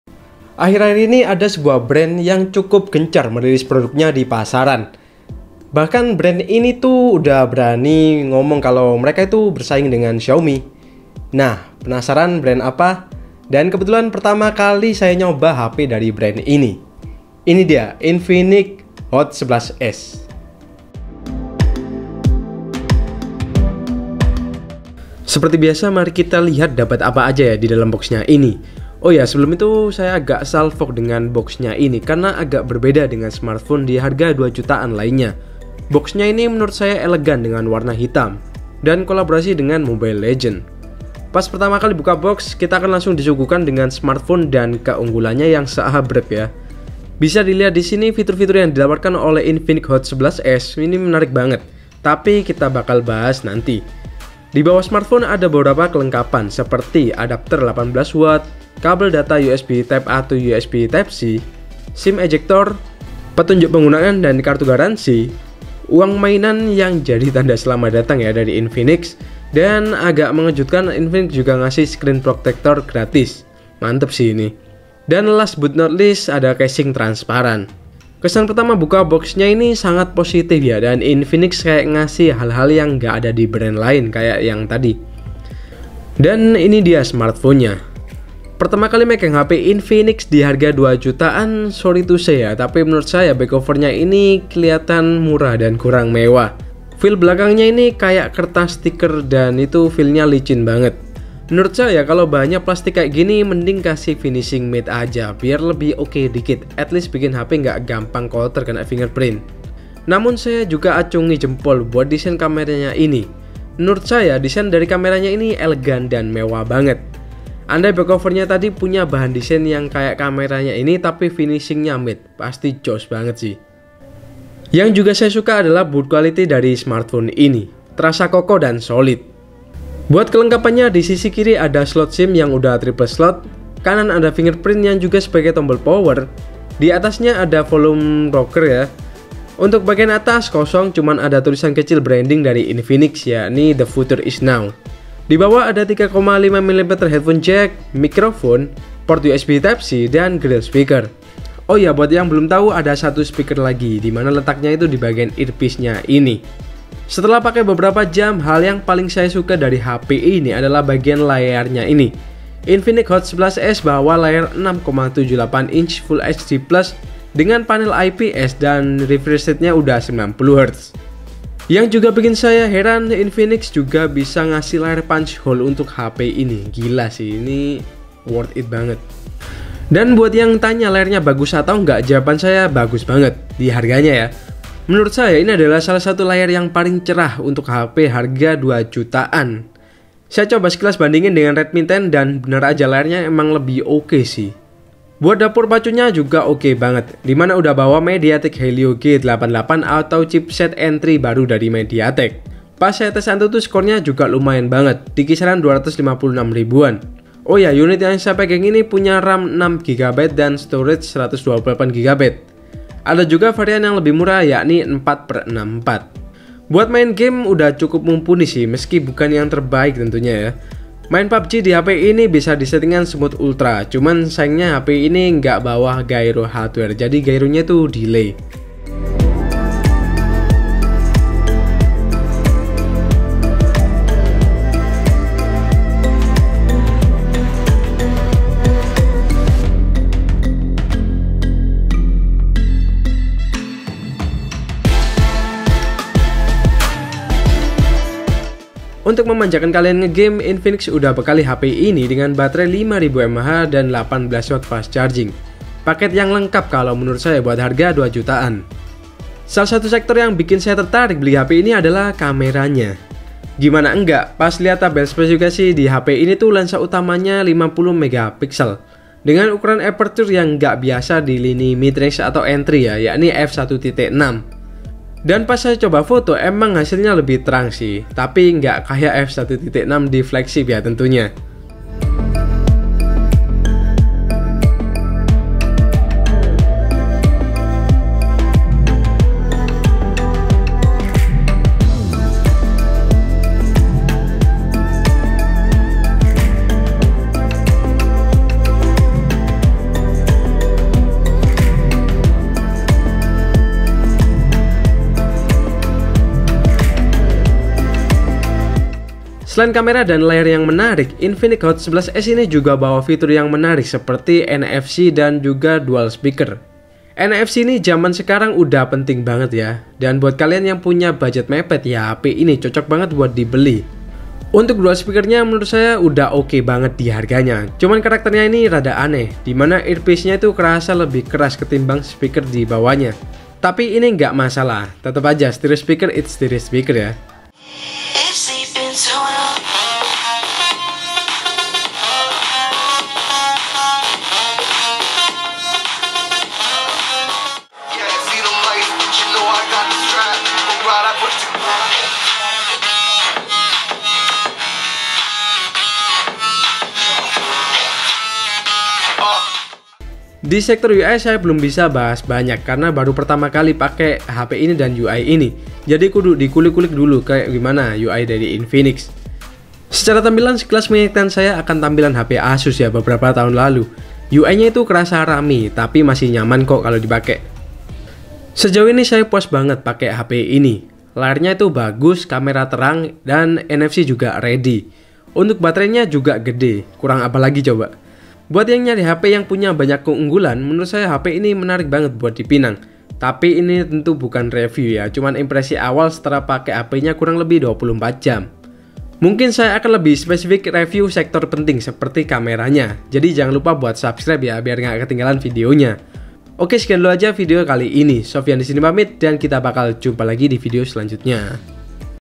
XIAOMI WAJIB WASPADA! | Infinix Hot 11s Indonesia
Infinix, pesaing Xiaomi urusan price to performance. Infinix Hot 11s ini salah satunya yang menarik. Gimana menurut kalian?

Tempat beli

Timestamp :
00:00 Opening
00:25 Unboxing
01:28 Bahas Desain
02:09 Kelengkapan Port
02:37 Bahas Display
03:15 Bahas Performa
03:36 Gaming Test
04:08 Bahas Baterai & Charging
04:19 Bahas Camera
04:41 Sample Camera
05:09 Bahas Fitur-Fitur
06:46 Penutup